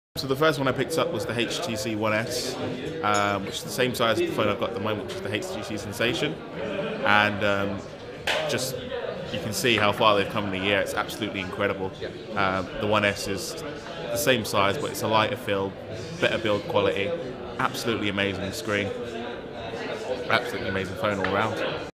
HTC One. As recommended by Cy. Manchester meet-up.
Мы взяли HTC One с собой в поездку по всему миру чтобы узнать что люди думают о нем. Это то, что один из гостей, Сью, сказал о своем опыте использования.